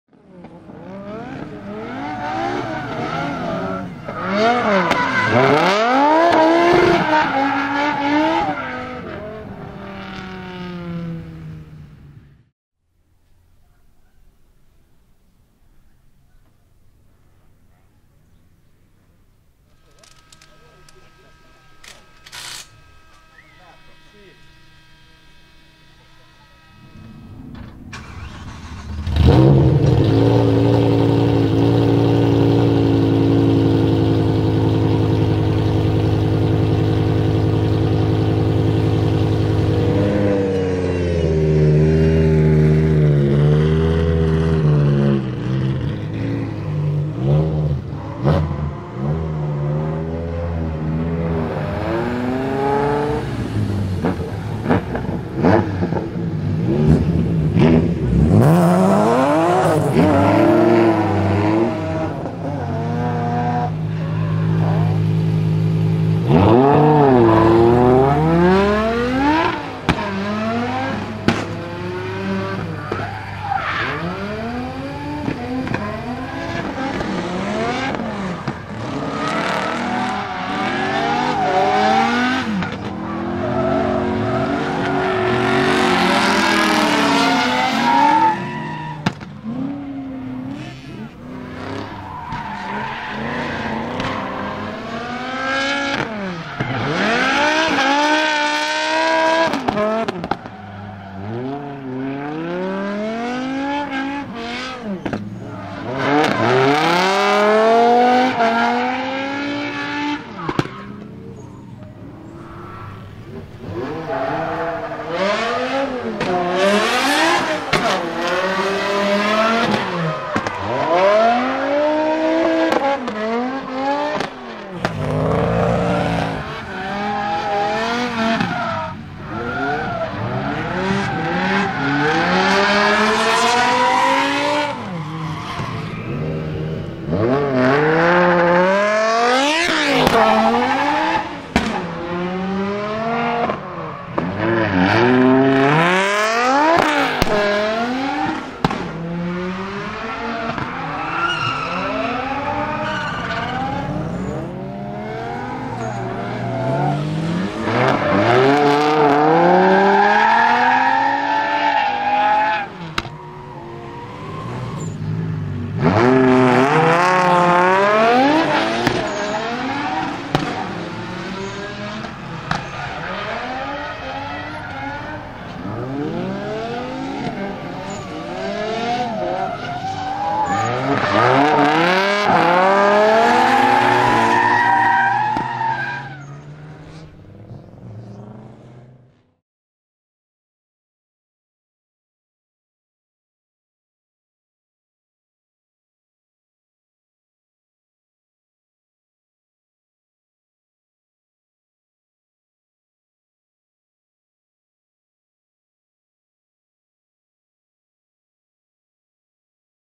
BMW E46 with M5 E60 V10 Engine Swap! - First Drifting Test ft. LOUD Backfires & Sounds!
First time on track for this BMW M3 E46 drift car powered by one of the world's best sounding engine ever!
I'm talking about the naturally aspirated 5.0-litre S85B50 V10 engine, introduced in the E60 M5, which is able to produce 500HP and 520Nm.

- Event: Drift Day
- Where: Ottobiano, Italy
- When: 5th September 2020